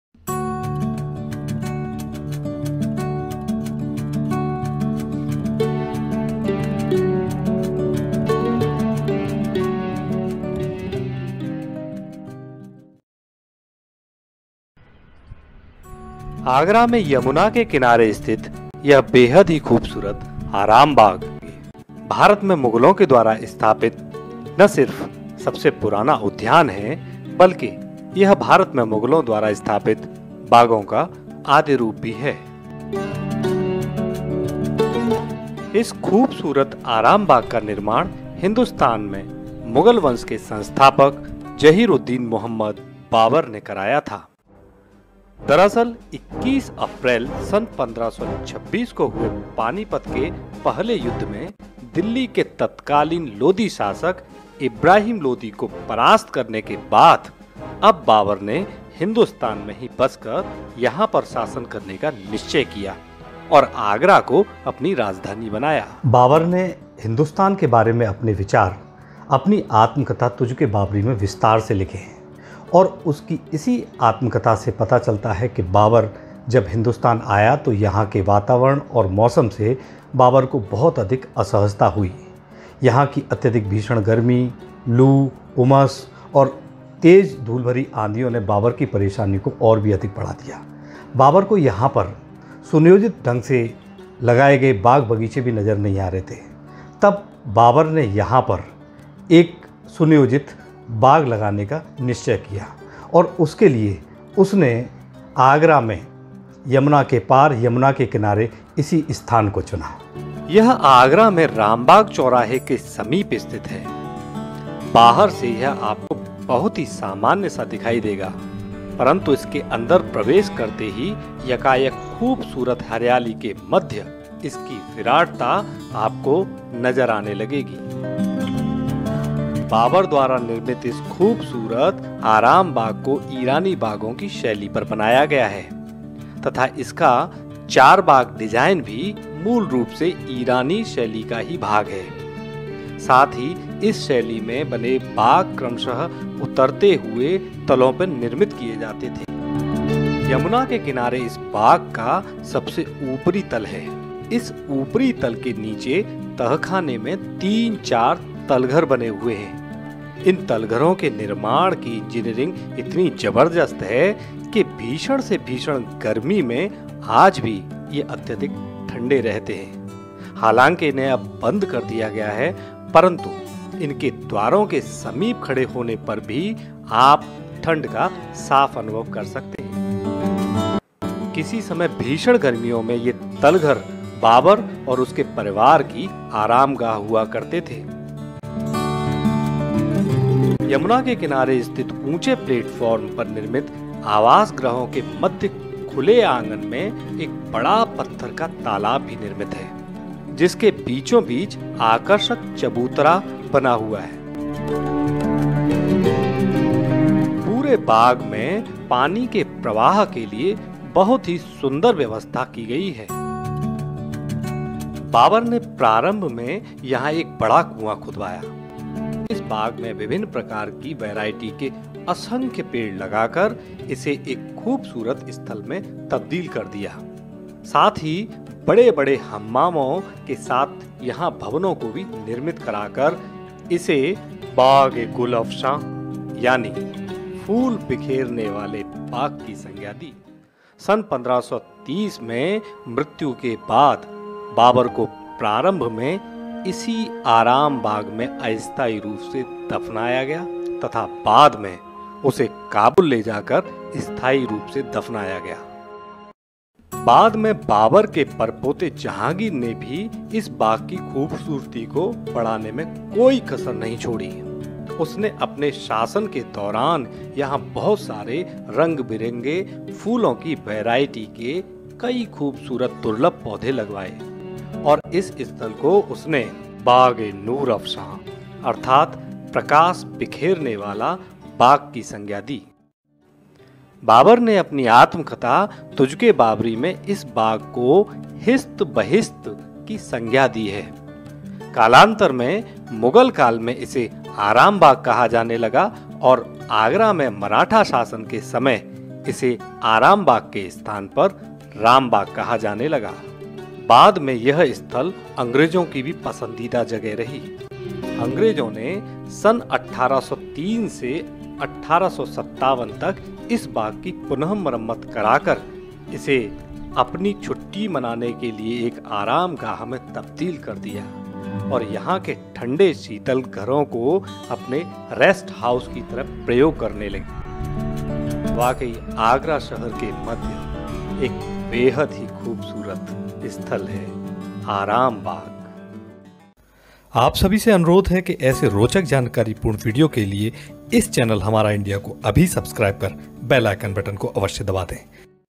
Aram Bagh(Ram Bagh) | आराम बाग आगरा | मुग़ल बादशाह बाबर का विश्राम स्थल | Agra
Aram Bagh(Ram Bagh) | आराम बाग आगरा | Directedby Shailendra Narwar |
आराम बाग आगरा में यमुना के किनारे स्थिति बेहद ही खूबसूरत स्थल है | यह भारत में मुगलों द्वारा स्थापित सबसे पुराने उद्यान के साथ साथ भारत में मुगलों द्वारा स्थापित बागों का आदिरूप भी है...
बाबर ने भारत के बारे में अपने अनुभव मूलतः तुर्की भाषा में लिखी अपनी आत्मकथा तुजुक ए बाबरी जो बाबरनामा के नाम से भी प्रसिद्ध है में लिखे हैं इसी पुस्तक से पता लगता है कि बाबर भारत के मौसम ने उसे बहुत सहज किया वह आगरा की भीषण गर्मी, उमस , धूल भरी तेज आंधीयों से बेहत ही परेशान हुआ उसे यहाँ अपने यहाँ के से बाग बगीचे नजर नहीं आए .अतः उसने यहां पर एक सुंदर प्रकृतिक बाग लगाने का निश्चय किया इसके लिए उसने आगरा में इस स्थान को चुना था ...